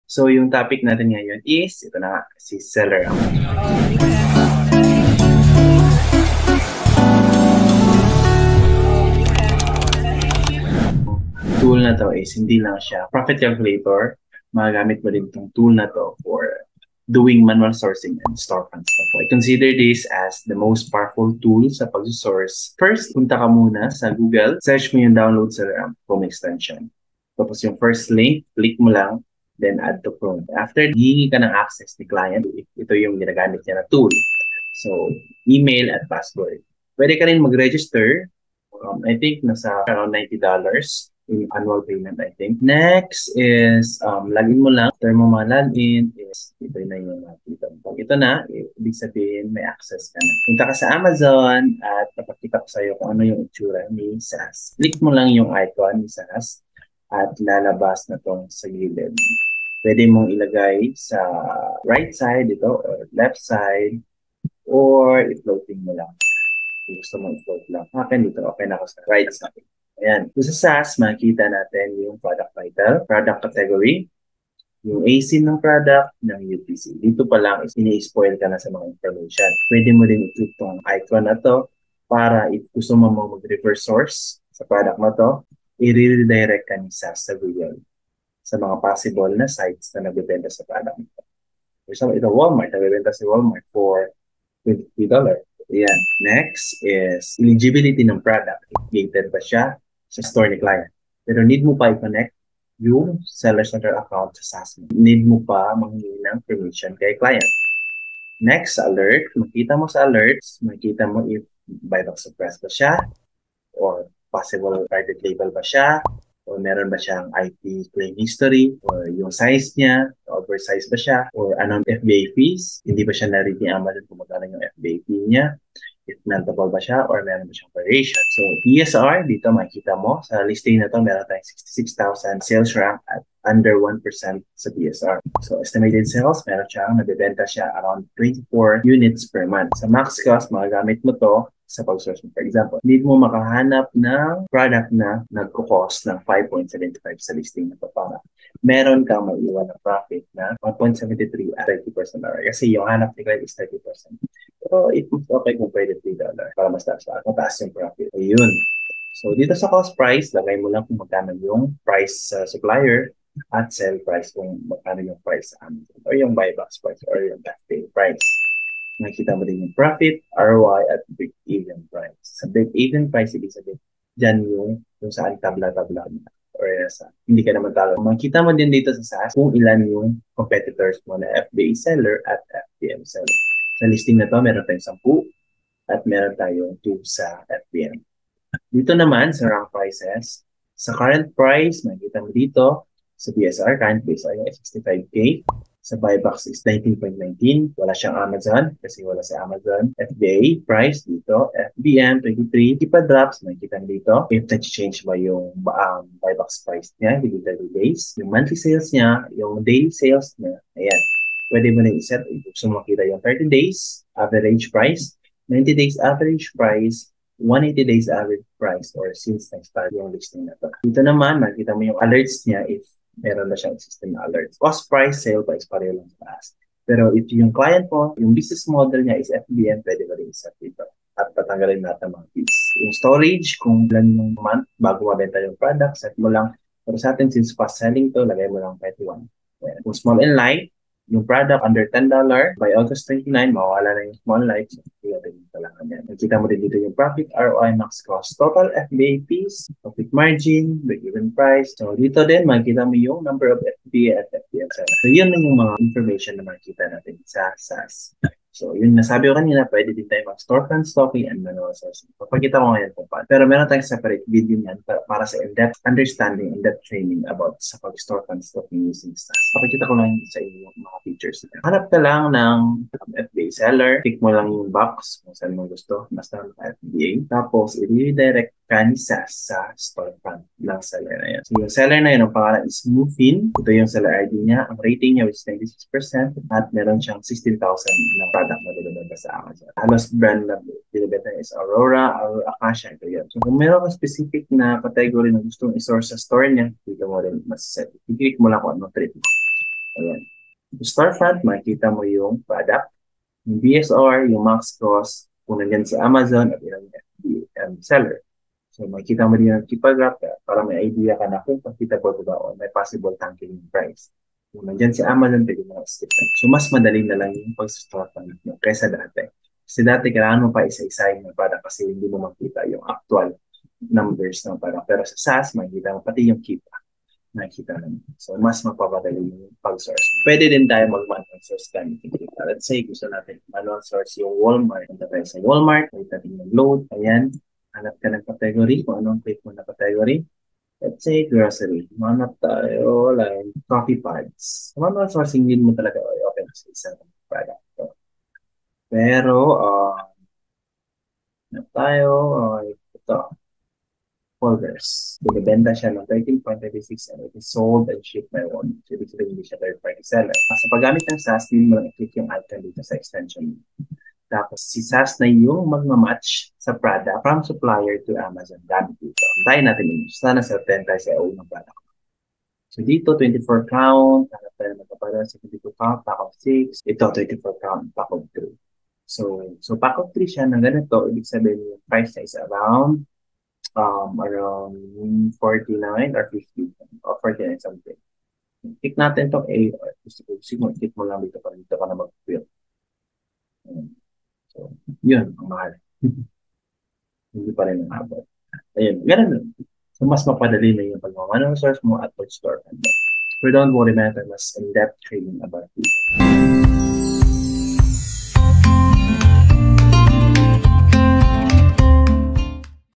Lesson 18: SellerAmp as a Profit Calculating Tool | Tagalog Discussion
🌐Lesson 20: Helium10 as a Profit Calculating Tool | Tagalog Discussion
The Amazon Product Researchers Hub:
Simplifying the PR Process, One video at a time!